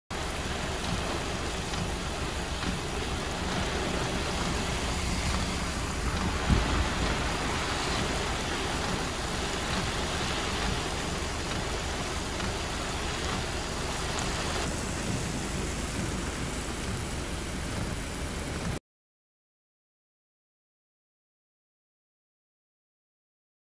Styrtregn i Årnes
Søndag ettermiddag traff uværet Årnes i Nes. Styrtregnet pågikk i noen i noen minutter.